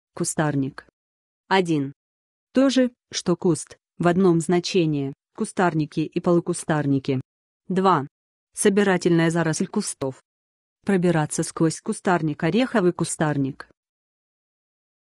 КУСТАРНИК - что это такое? значение и описание
Хочешь знать что значит КУСТАРНИК? тогда слушай до конца. n n -а, м. 1. То же, что куст (в 1 знач.) (спец.). Кустарники и полукустарники. 2. собир. Заросль кустов. Пробираться сквозь к. Ореховый к. || уменьш. кустарничек, -чка, м. || прил. кустарниковый, -ая, -ое. Кустарниковые растения. Кустарниковые заросли. n n Больше слов